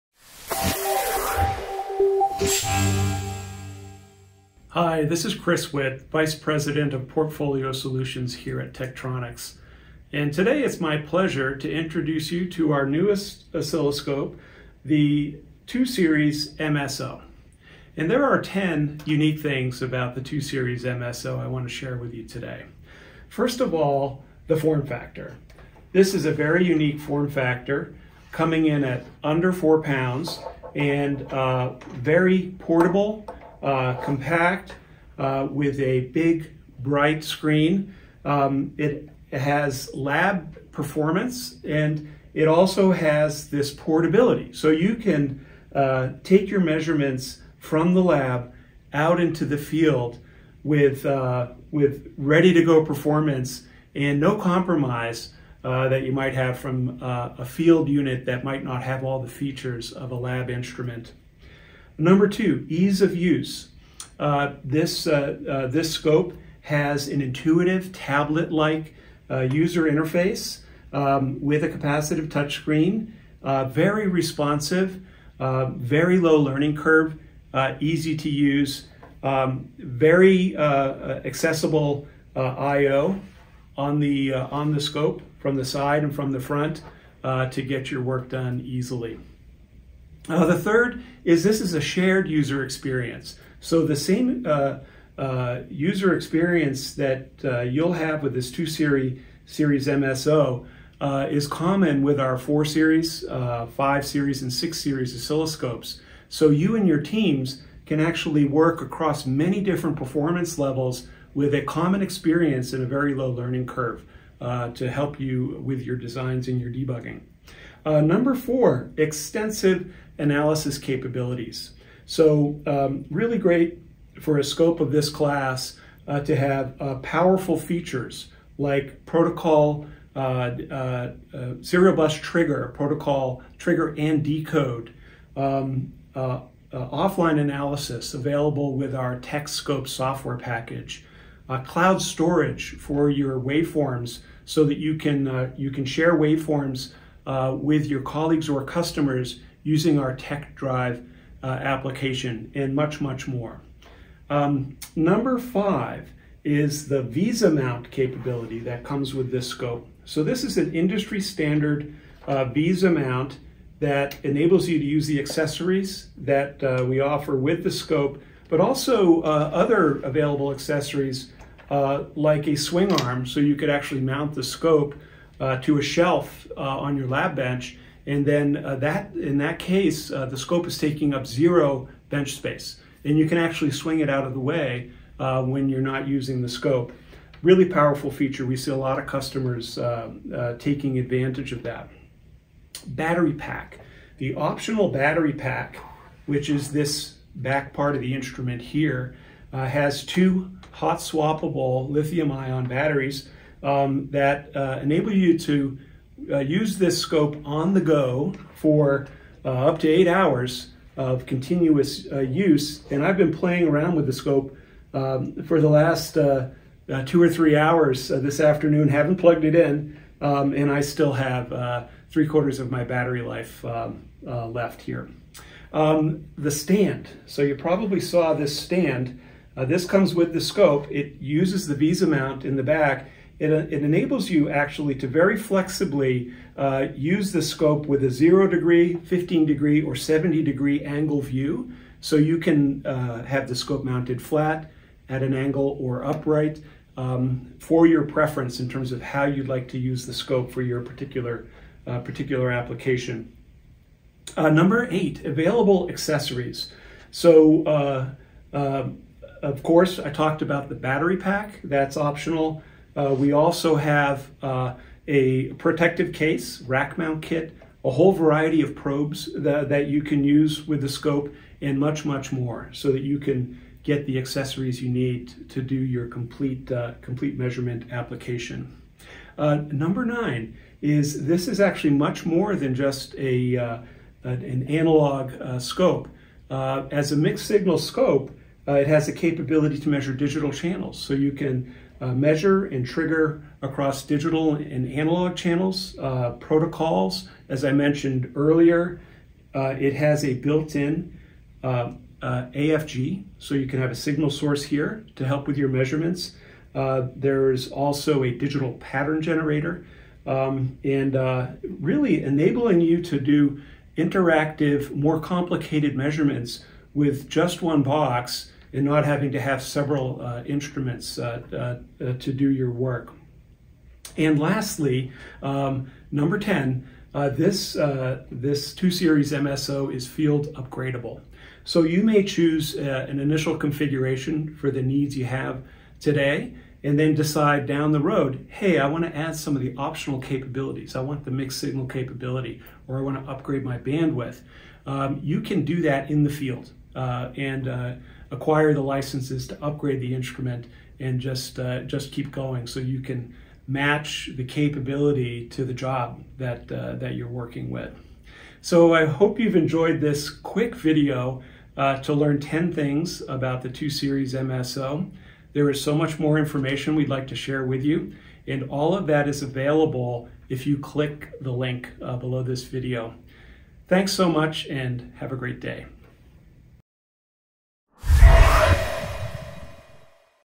2 Series MSO Demo with Chris Witt
Chris Witt, VP of Tektronix portfolio solutions walks you through the features of this small but mighty 2 Series Mixed Signal Oscilloscope.

Learn more about the 2 Series MSO by clicking the